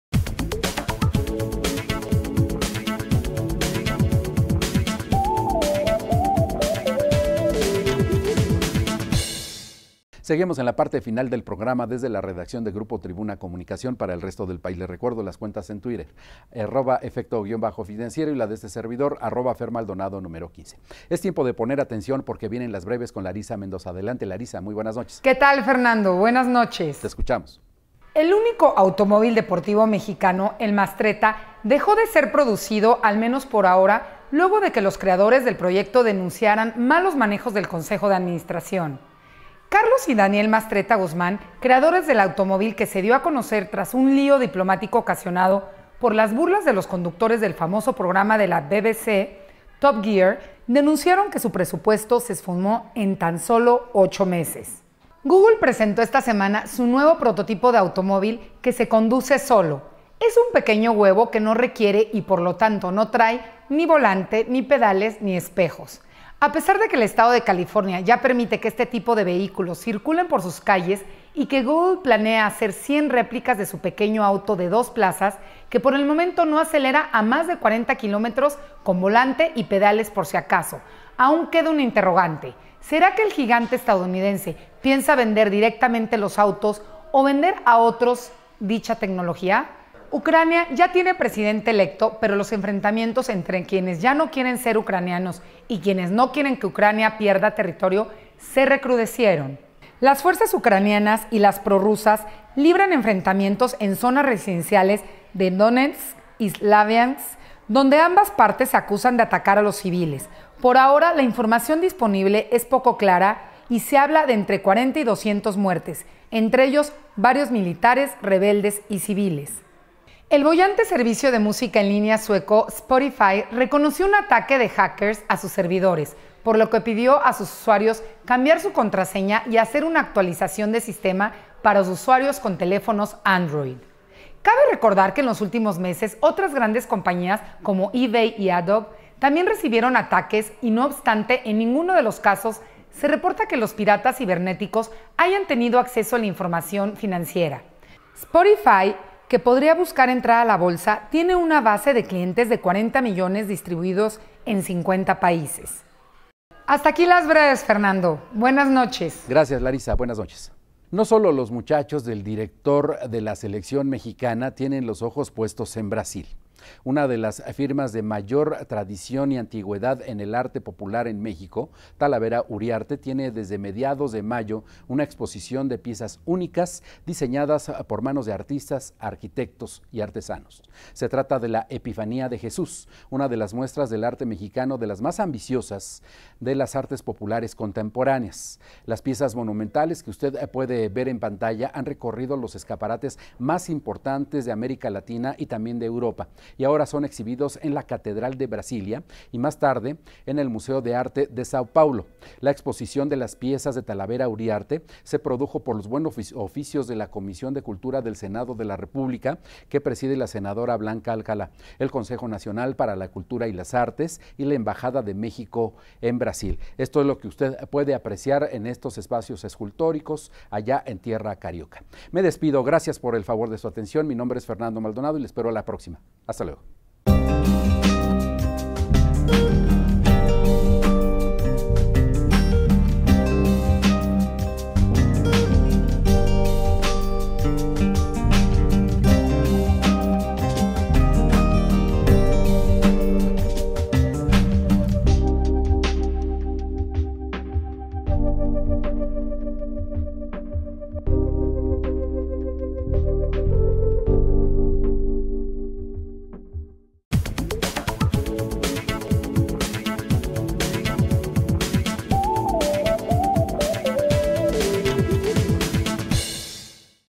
Breves con Larissa Mendoza Efekto financiero
Autos veloces, para cualquier comodidad como el nuevo coche que no trae volante, pedales y espejos. El carro llamado huevo, en California ya puede circular por sus calles y que Google piensa hacer 100 ejemplares.